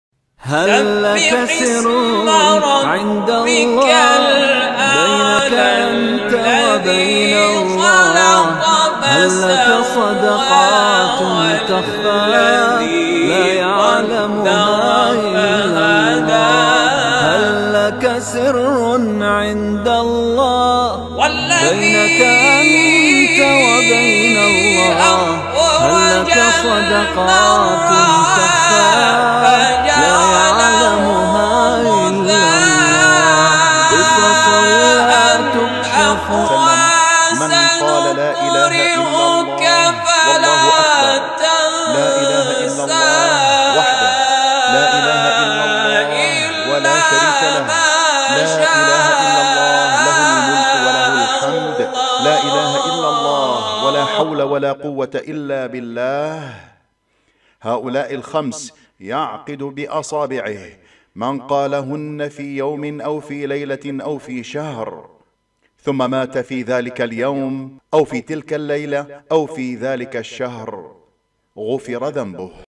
بلماضي إماما للاعبين فور وصولهم الكاميرون...هذا هو المنتخب الذين نريده دين ودنيا
بلماضي لاتروخ عليه لاصغيرة ولاكبيرة يهتم بكل شئ